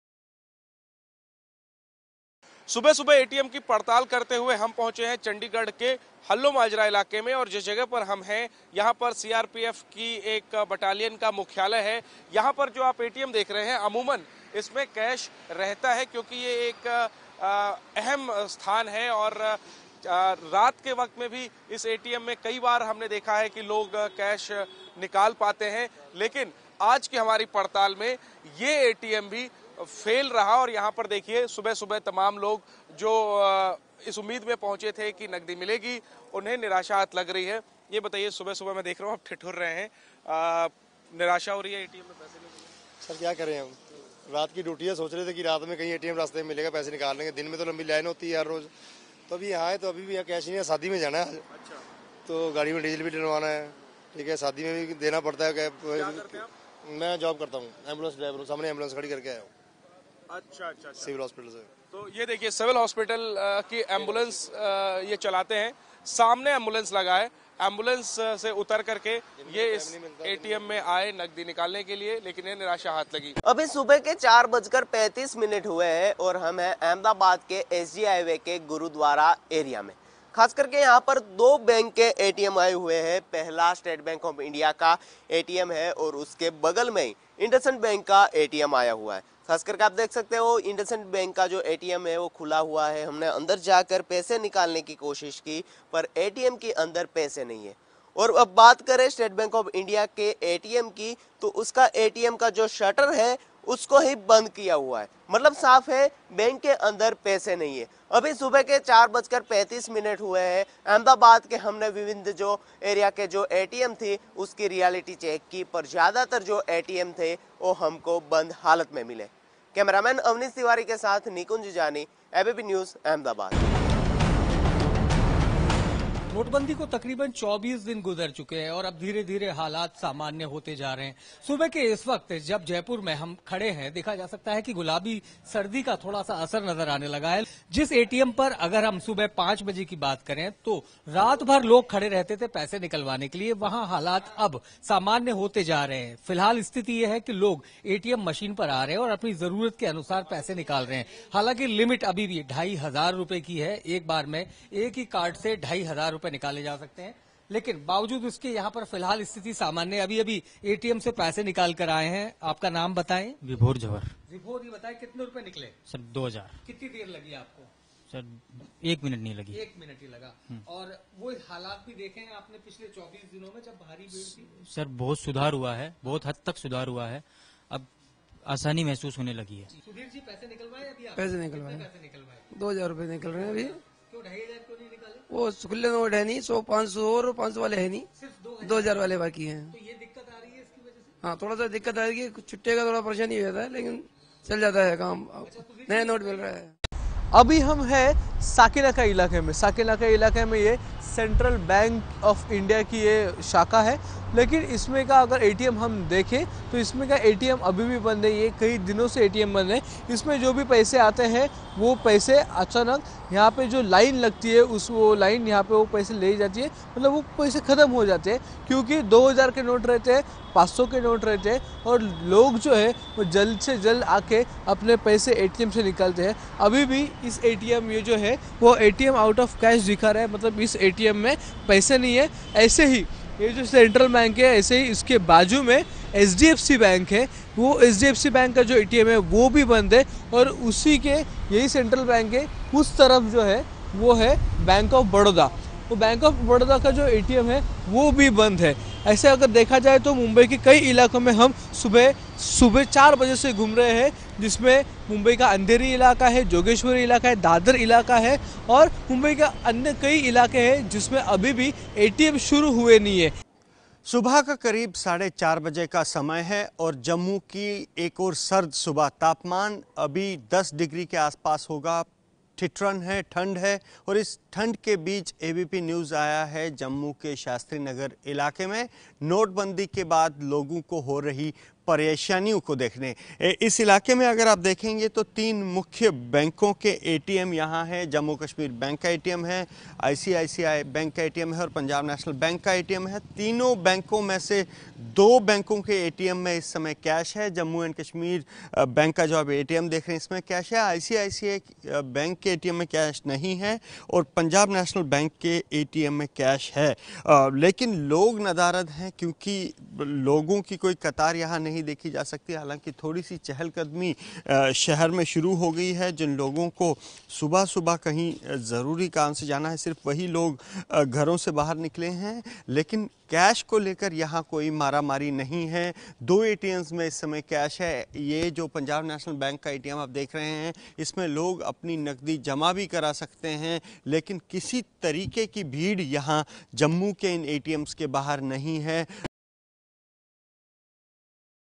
People still waiting for cash outside ATMs in Chandigarh
People still waiting for cash outside ATMs in ChandigarhFor latest breaking news, other top stories log